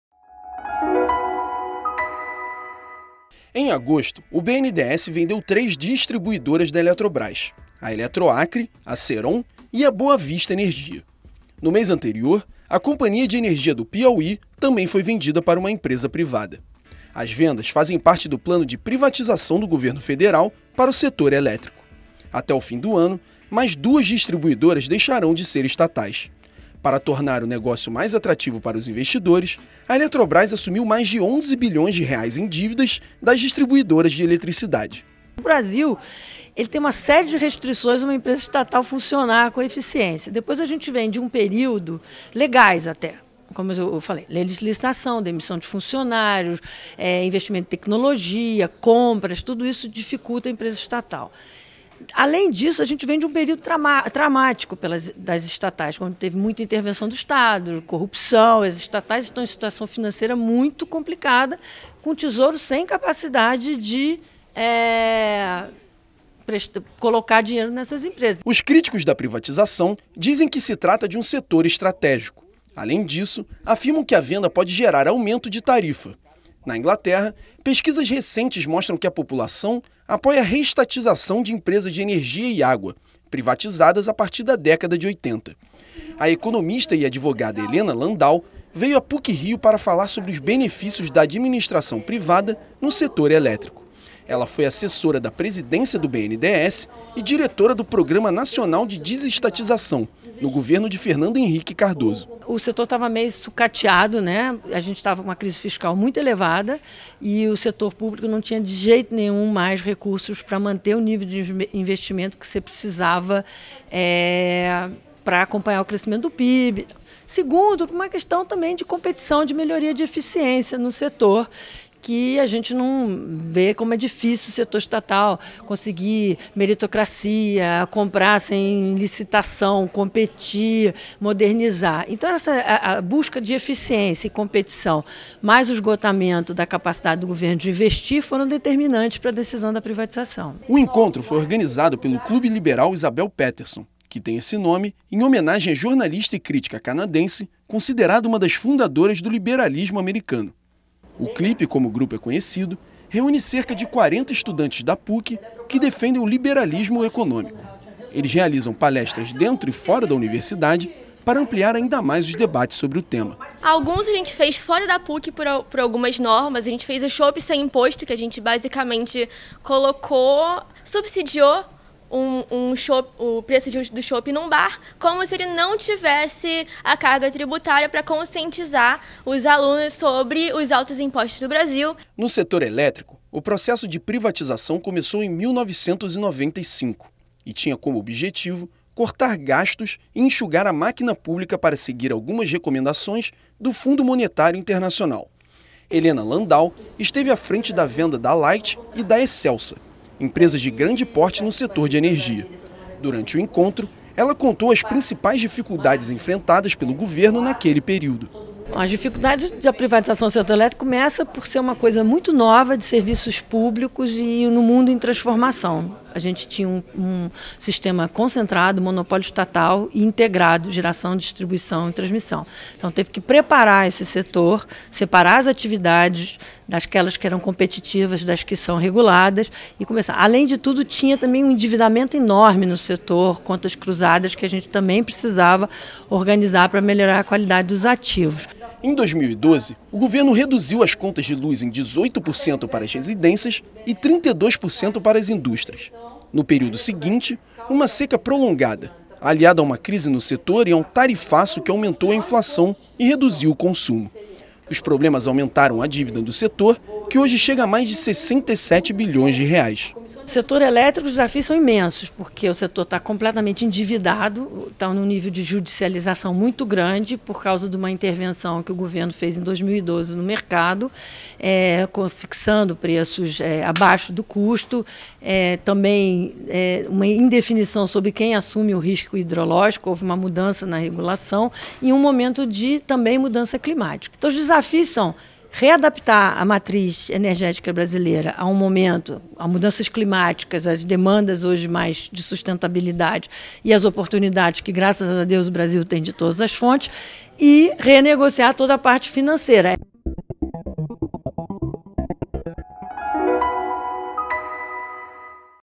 TV PUC-Rio: Elena Landau defende privatização do setor elétrico
O Governo Federal iniciou a privatização do setor elétrico, que já vendeu três distribuidoras da Eletrobras este ano e deve leiloar outras duas empresas até dezembro. A economista Elena Landau veio à PUC-Rio para falar sobre os benefícios dessas privatizações.